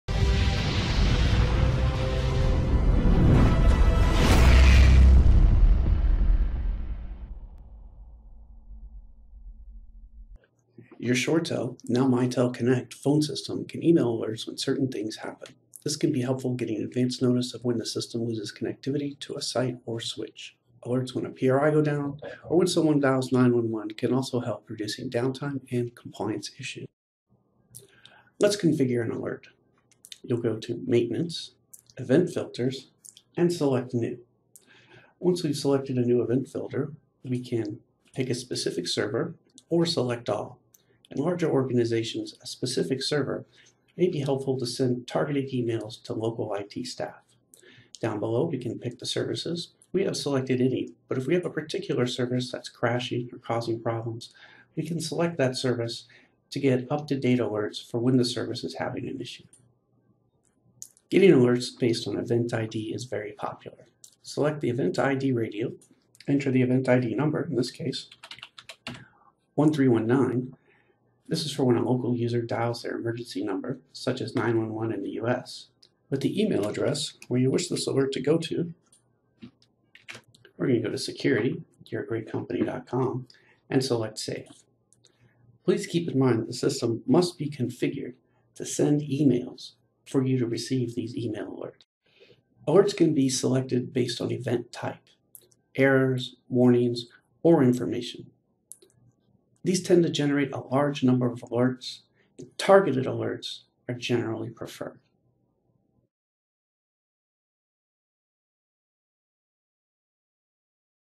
How to set up automated alerts in Mitel (ShoreTel Connect)
How to set up automated email alerts for different types of events in MiCloud Connect (ShoreTel Connect) Director. Video instruction for a system administrator.